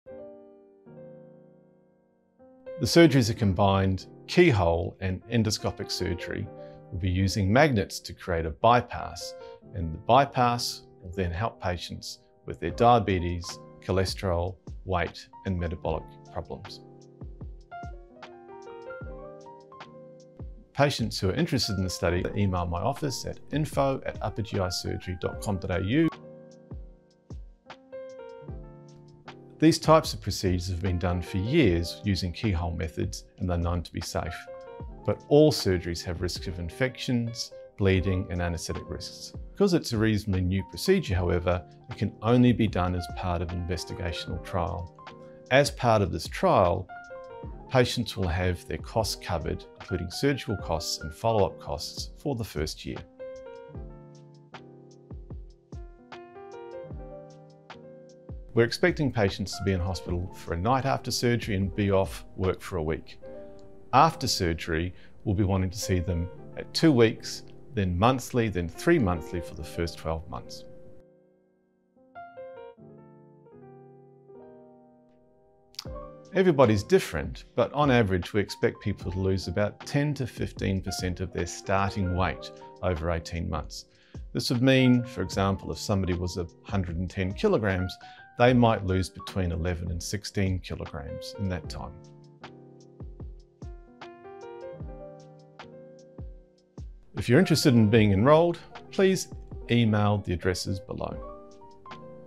MagDI Australia Study
Creation of Side-to-Side Compression Anastomosis Using the GT MagDI System in Australia to Achieve Duodeno-Ileal Diversion in Adults with Obesity and with or without Type 2 Diabetes Mellitus (MagDI Australia Study)